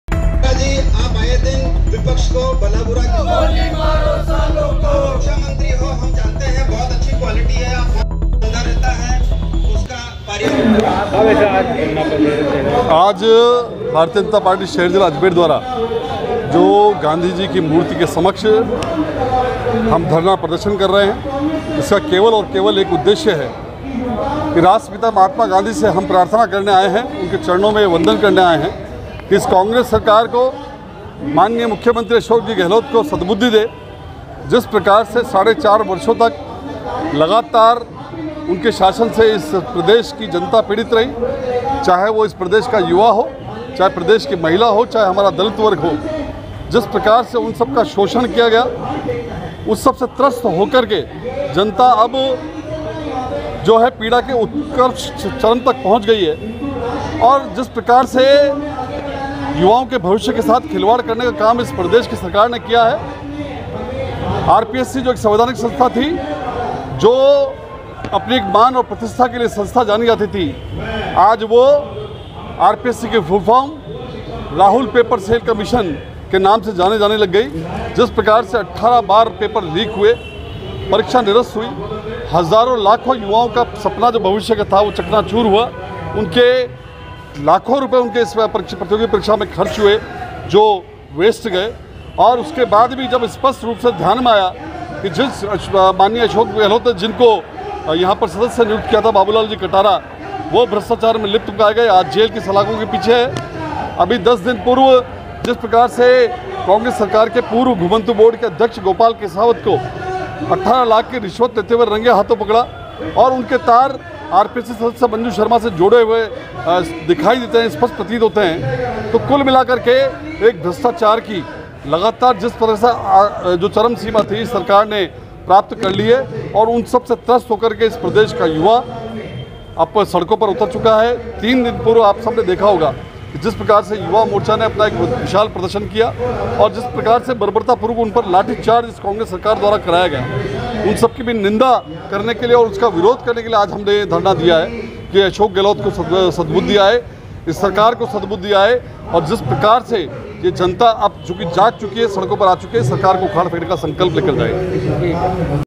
Nahisahegarajasthan अभियान के तहत अजमेर में प्रदर्शन
भारतीय जनता पार्टी शहर अजमेर द्वारा अब नहीं सहेगा राजस्थान अभियान के तहत धरना प्रदर्शन